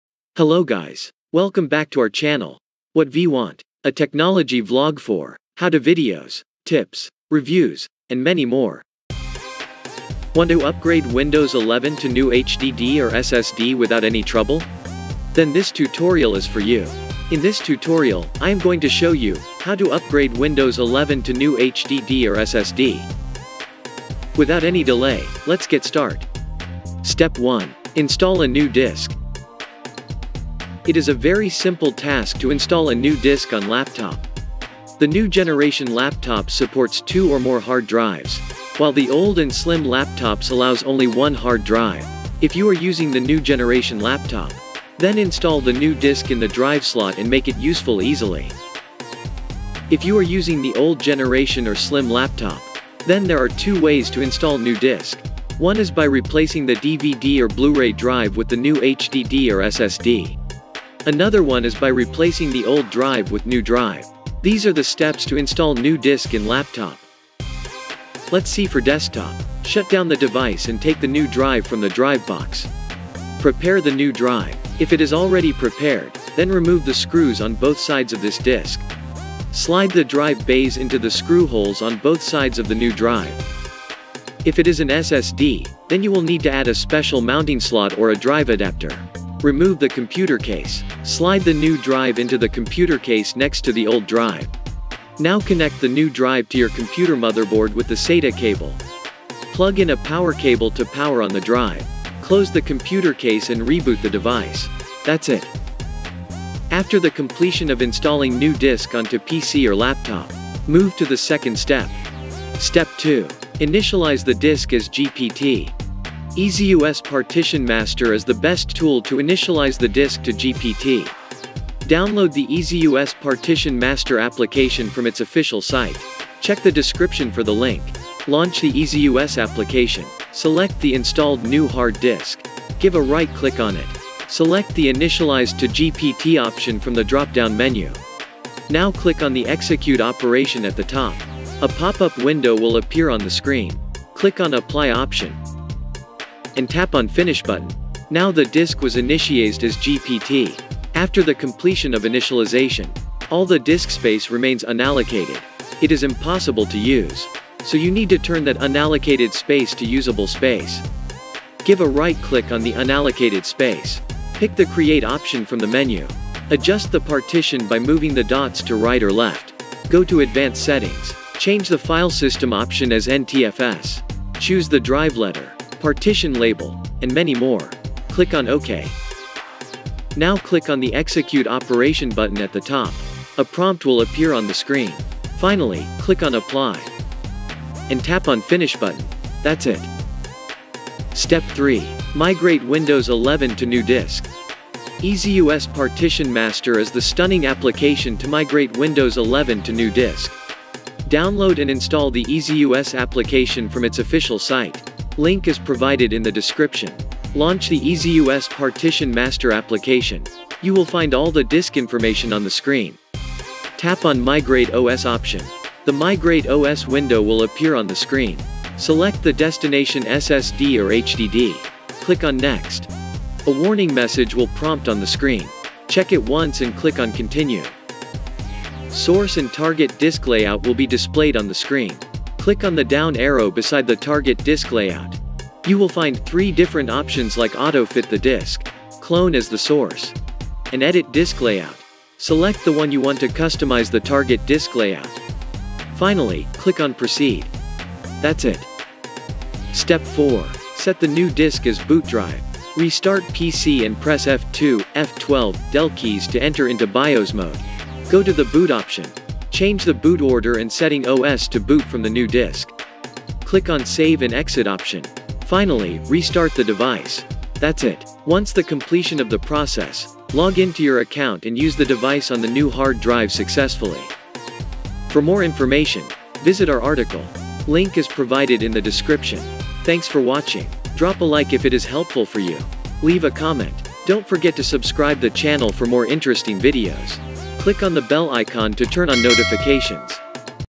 How to upgrade Windows 11 to New HDD or SSD?
In this tutorial, you will find all information about installing a new disk, initializing the disk, migrating OS, and many more. It helps you to upgrade your old disk with the new one easily.
0:00 - Channel Intro
0:09 - Topic Intro
0:27 - 1)Install New Disk
1:52 - 2)Initialize the disk as GPT
3:11 - 3)Migrate Windows 11 to new Disk
4:10 - 4)Set the new disk as Boot Drive